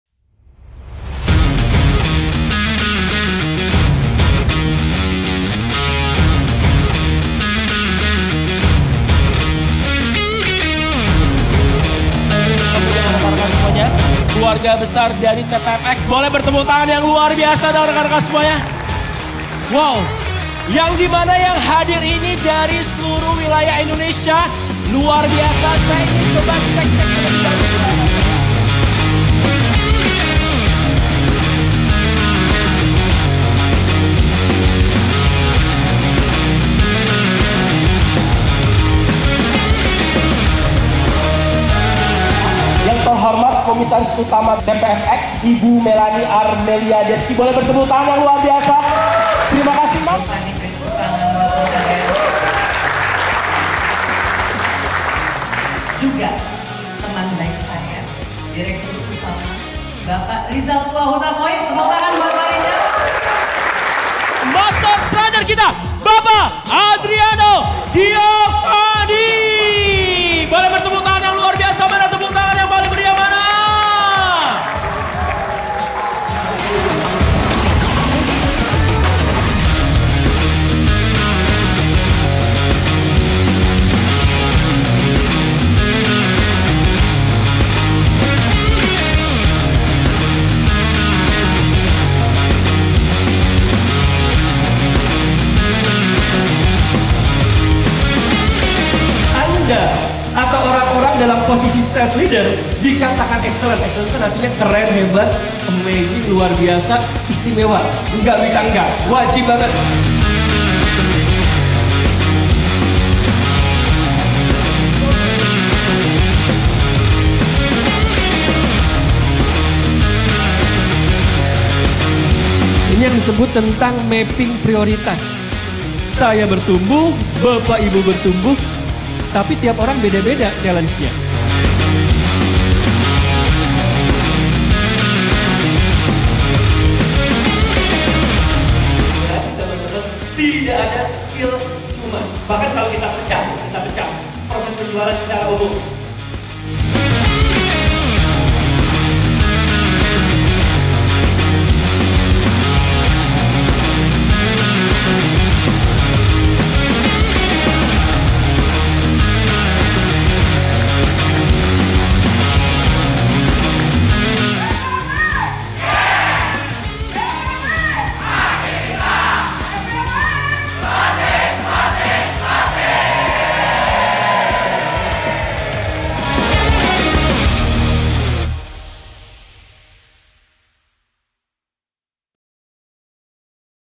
Be An Excellent Sales Leader | TPFx Indonesia
Hallo Sobat TPFx

Selamat datang di video after event seminar TPFx Indonesia dengan tema "Be an Excellent Sales Leader" yang dilaksanakan pada 19 Juni 2024. Acara ini bertujuan untuk meningkatkan kualitas tim marketing TPFx Indonesia dengan memberikan pengetahuan dan strategi terbaru dalam kepemimpinan penjualan. Seminar ini juga diadakan untuk memotivasi tim agar lebih produktif, berinovasi, dan mampu mencapai target dengan lebih efektif. Selamat menyaksikan!